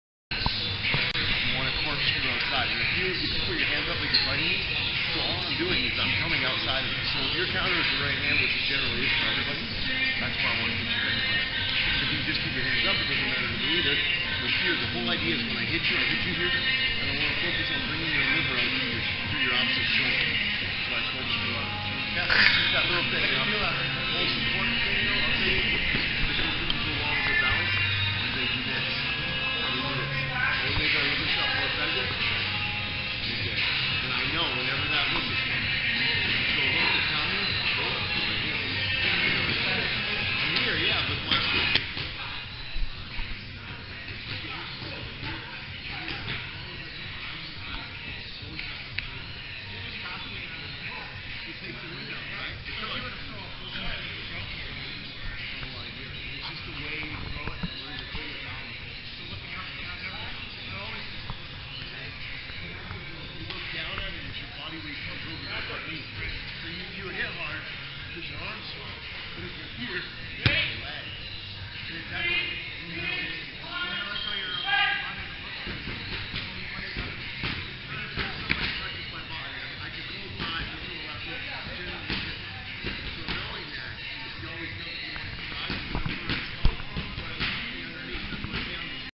Liver Shot Technique Shawn Tompkins
Training video from Vegas 2012. Learning the liver shot from Shawn Tompkins.